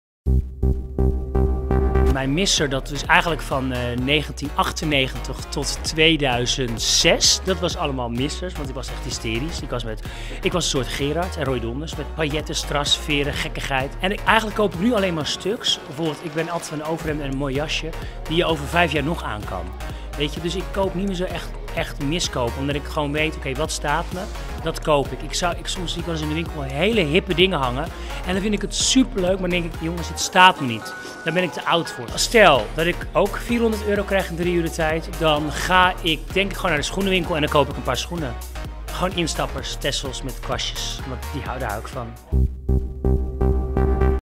De modemissers van Fred van Leer - Superguide - Veronica Magazine
In Shopping Queens VIPS krijgen kandidaten 400 euro zakgeld om in drie uur tijd een outfit te scoren, terwijl anderen hun kledingkast beoordelen. Fred van Leer, presentator en stylist, staat natuurlijk bekend om zijn ongezouten commentaar op de kledingstijl van anderen. Zelf ziet hij er vaak op en top uit, maar ook hij heeft toch echt wel eens de plank misgeslagen.

Superguide vroeg hem hiernaar, waarop een verrassend antwoord kwam: "Mijn misser was eigenlijk van 1998 tot 2006, ik was een soort Gerard, en Roy Donders." Ook wilden we natuurlijk weten wat híj als eerste zou kopen als hij 400 euro zakgeld meekreeg!